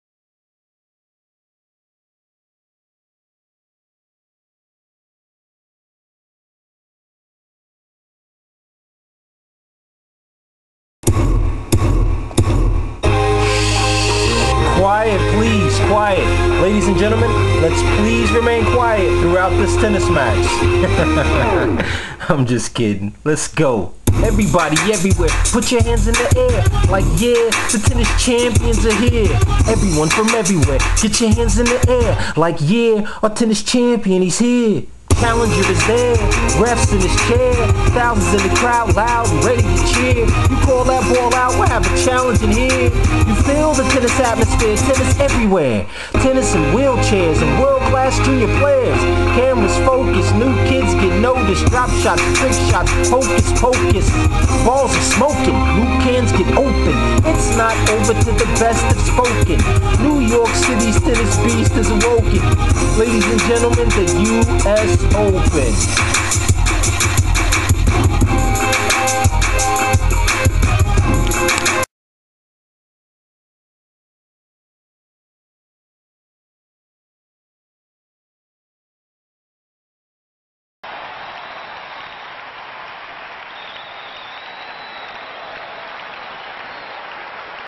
US OPEN [TENNIS] MUSIC VIDEO!
A BIG thanks for my tennis coach for giving me the song...we turned out a great US Open Tennis Video. Enjoy!